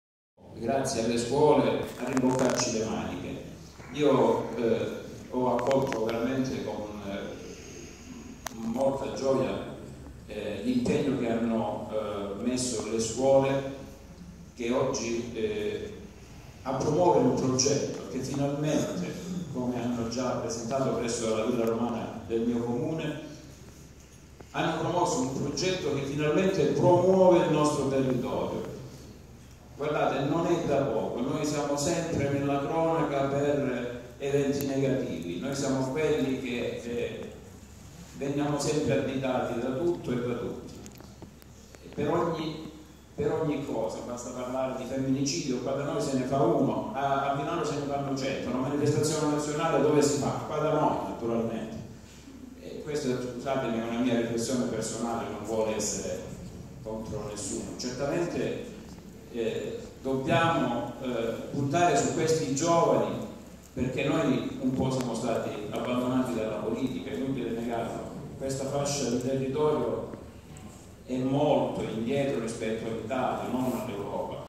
9 Maggio 2017/ASOC201617:"Festa dell'Europa".
L'evento si è tenuto presso il Palazzo della Cultura di Locri - Rc.